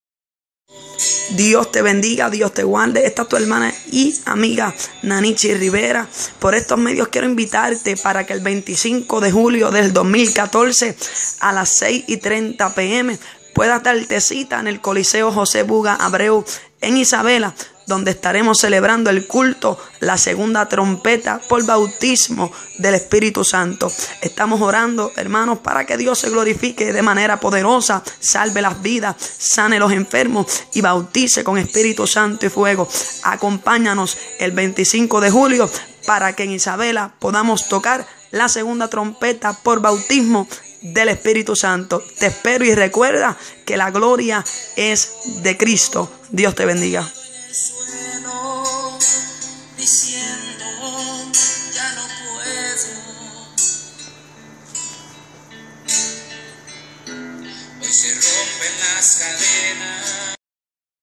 Evang. Nanichy Rivera - 25 de julio 2014 Segunda trompeta
ESTE 25 DE JULIO EN EL COLISEO JOSE BUGA ABREU EN ISABELA ESTAREMOS CELEBRANDO EL CULTO DE LA SEGUNDA TROMPETA POR BAUTISMO DEL ESPIRITU SANTO.
La gloria al CRISTO DE LA GLORIA,